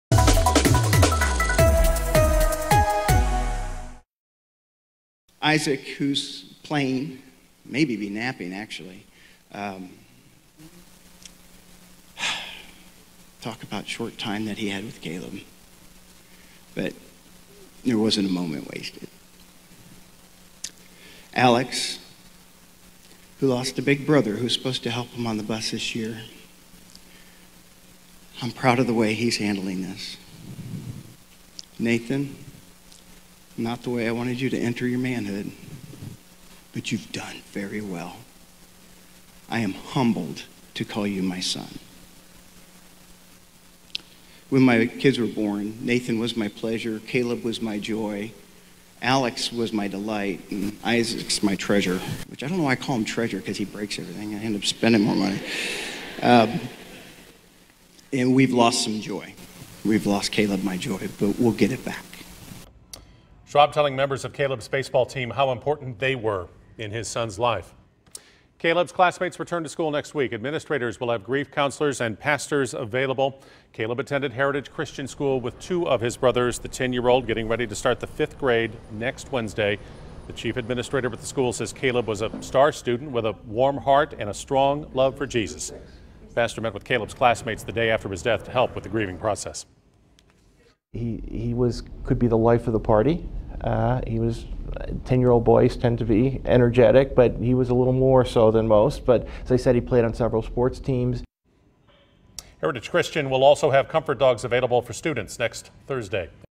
Friends, family remember boy killed while riding Schlitterbahn water slide
Friends and family gather Friday to remember a boy who died while riding a Schlitterbahn water slide.